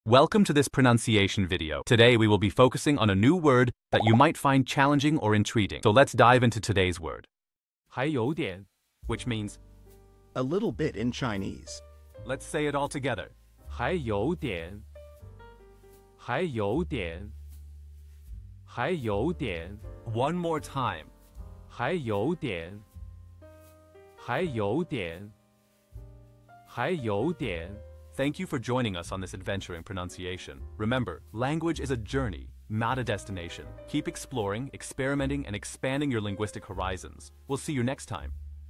How to pronounce hái yǒu diǎn | 還有點 (A little bit in Chinese)
Master the Pronunciation of 'hái yǒu diǎn | 還有點' - which means : A little bit in Chinese 📝 with @PerfectPronunciationChinese 🎓🗣️ - [Your Guide to Chinese]

Learn to pronounce 'hái yǒu diǎn' perfectly in Chinese with our easy and engaging tutorial by @PerfectPronunciationChinese! 📖📽️ In this comprehensive video, we provide you with native speaker examples 🗣️👂 and phonetic breakdowns 🎵 to ensure you get the pronunciation just right.